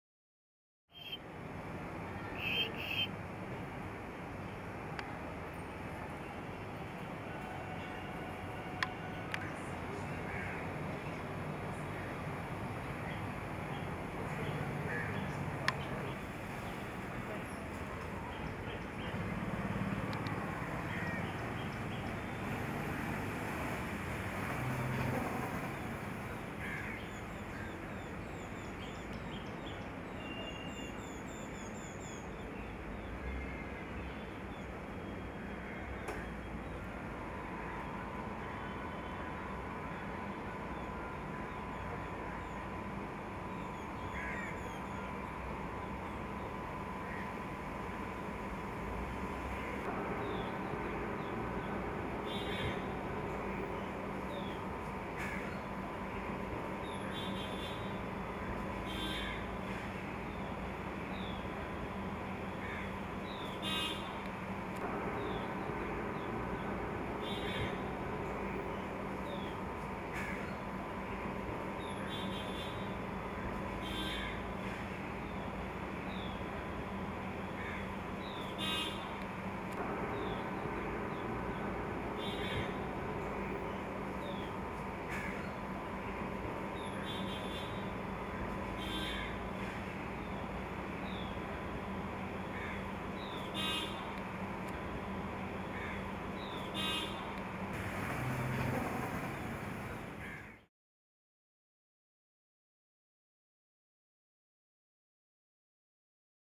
Cat hiding over a tent tries to get some sleep
Sitting on a green net in the parking lot of Wildfilmsindia office, this cute little cat tries to catch some sleep. The cat gets distracted by the presence of the cameraman forcing it to focus its energy in parking.